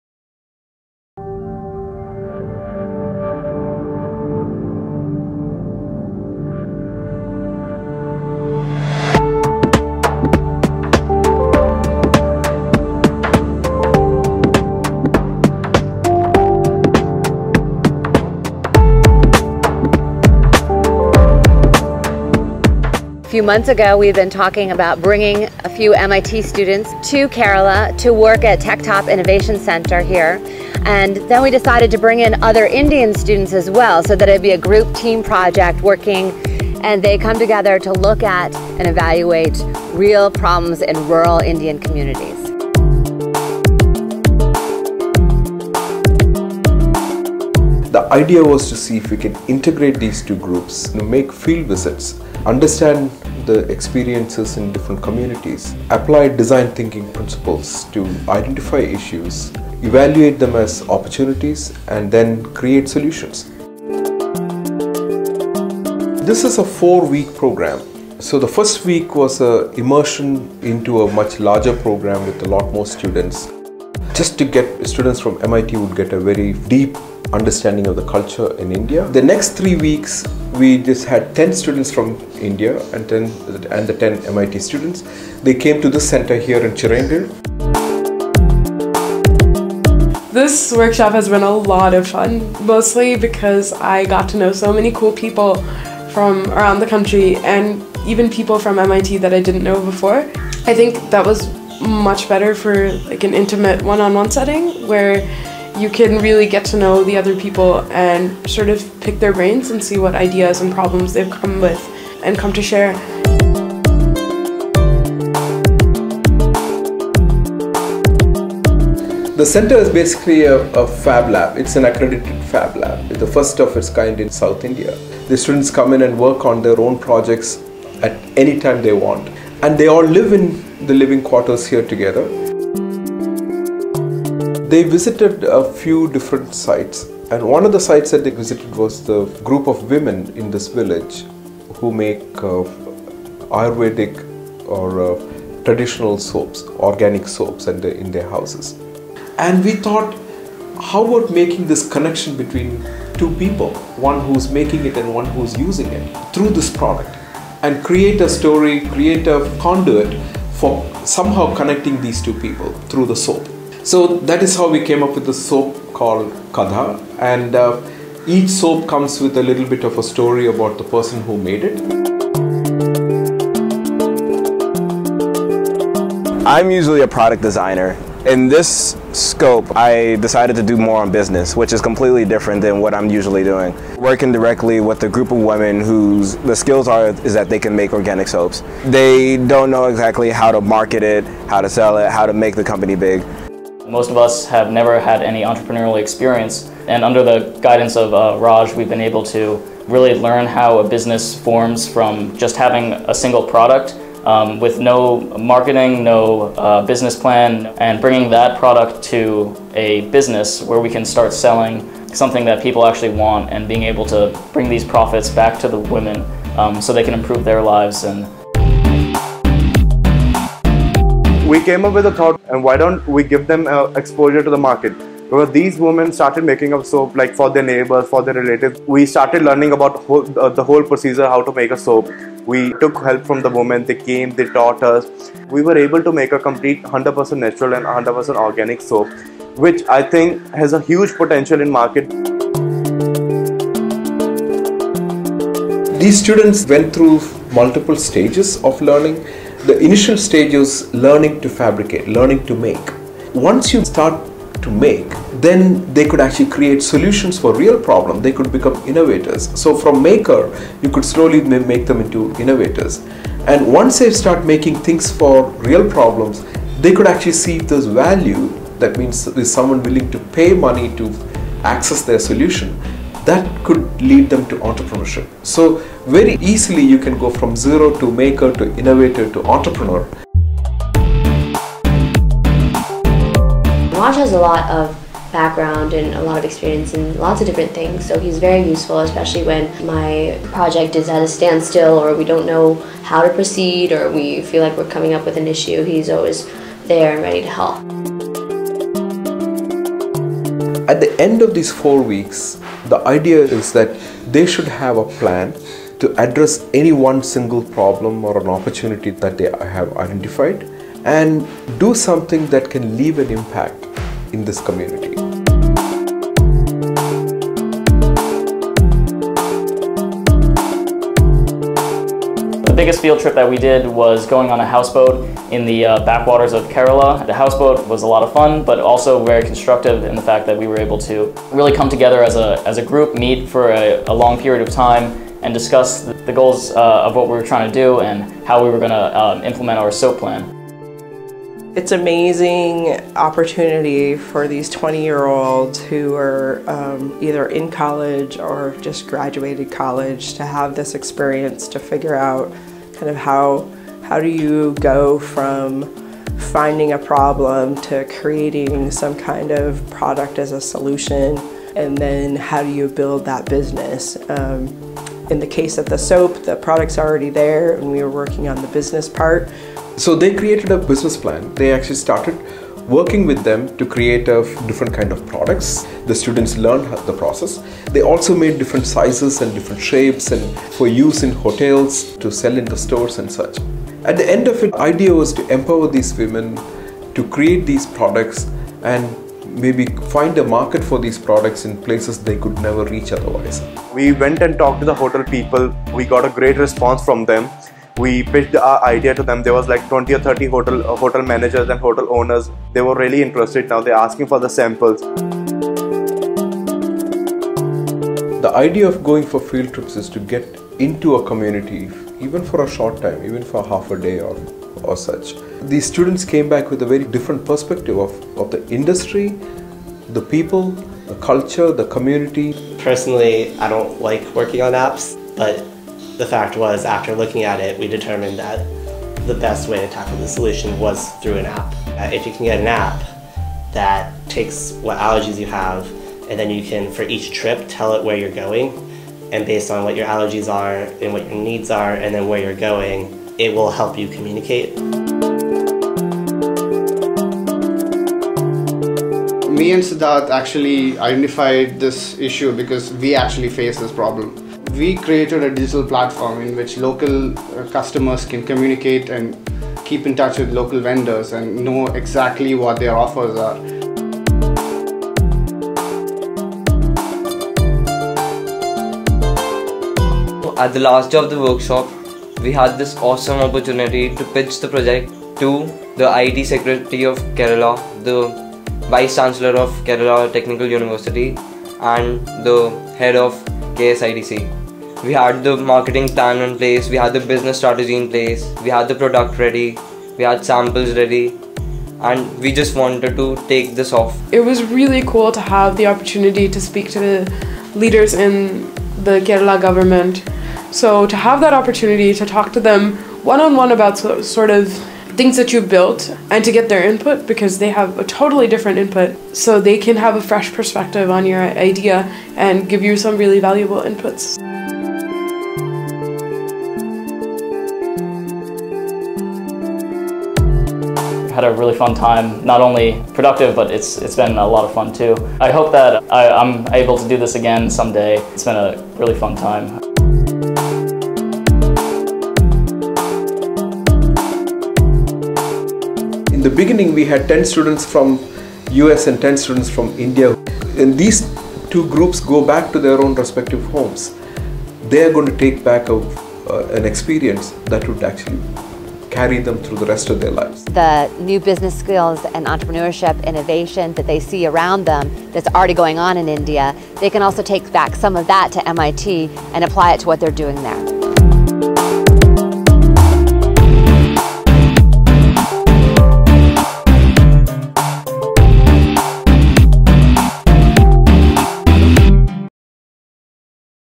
MIT Make In India | TechTop 2015 | Rajesh Nair
Ten students from the Massachusetts Institute of Technology and ten from India came together to Chirayinkil, a village in Kerala for an experiment to see if any student can be trained in innovation and entrepreneurship.

One month-long workshop was attended by 20 Participants

It's a short movie clip of the workshop conducted in South India in 2015.